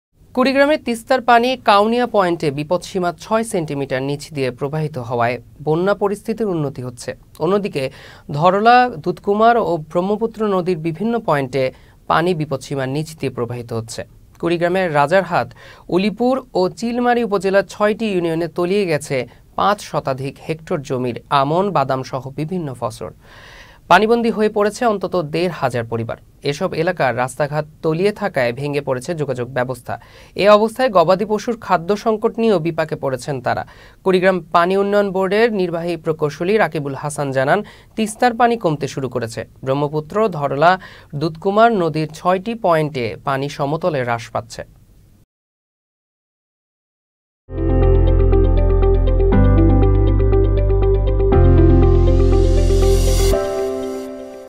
কুড়িগ্রামে তিস্তার পানি কাউনিয়া পয়েন্টে বিপদসীমার নীচ দিয়ে প্রবাহিত হচ্ছে | Channel S News
কুড়িগ্রামে তিস্তার পানি কাউনিয়া পয়েন্টে  বিপদসীমার ৬ সেন্টিমিটার নীচ দিয়ে প্রবাহিত হওয়ায়, বন্যা পরিস্থিতির উন্নতি হচ্ছে। অন্যদিকে, ধরলা,দুধকুমার ও ব্রহ্মপুত্র নদীর বিভিন্ন পয়েন্টে পানি বিপদসীমার নীচ দিয়ে প্রবাহিত হচ্ছে।
কুড়িগ্রামের রাজারহাট,উলিপুর ও চিলমারী উপজেলার ৬ টি ইউনিয়নে তলিয়ে গেছে, ৫ শতাধিক হেক্টর জমির আমন, বাদামসহ বিভিন্ন ফসল। পানিবন্দী হয়ে পড়েছে অন্তত দেড় হাজার পরিবার। এসব এলাকার রাস্তা-ঘাট তলিয়ে থাকায় ভেঙ্গে পড়েছে যোগাযোগ ব্যবস্থা। এ অবস্থায় গবাদিপশুর খাদ্য সংকট নিয়েও বিপাকে পড়েছেন তারা। কুড়িগ্রাম পানি উন্নয়ন বোর্ডের নির্বাহী প্রকৌশলী রাকিবুল হাসান জানান,তিস্তার পানি কমতে শুরু করেছে। ব্রহ্মপুত্র, ধরলা, দুধকুমার নদীর ৬টি পয়েন্টে পানি সমতলে হ্রাস পাচ্ছে.

1: News :
2: Entertainment News :
3: Sports News :